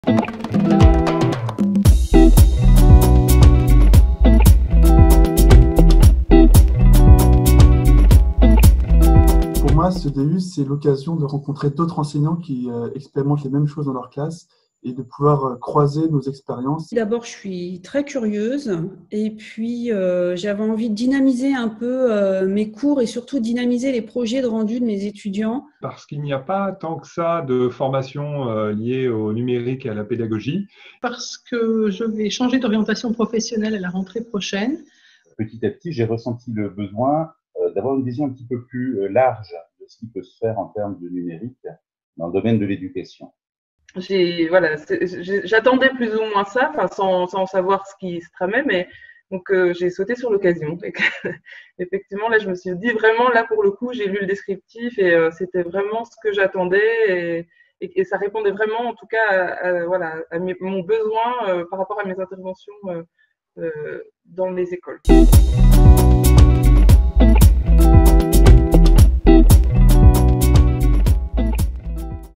Choisir le D.U "Usages du numérique en éducation"
Quel sens donner au numérique dans ses pratiques pédagogiques ? Comment préparer ses cours avec le numérique ? Comment l’intégrer dans ses pratiques de classe ? Autant de questions dont il convient aujourd’hui de se saisir afin de faire évoluer sa réflexion et ses pratiques éducatives.

Les compétences et connaissances développées au sein du DU "Usages du numérique en éducation" s’articulent autour des objectifs suivants :
- Acquérir des compétences techniques élémentaires
- Faire de la veille
- Découvrir ce qu’est le multimédia
- Développer une expertise numérique
- Intégrer le numérique dans sa pratique de classe
- Travailler en groupe et collaboration
- Gérer les identités numériques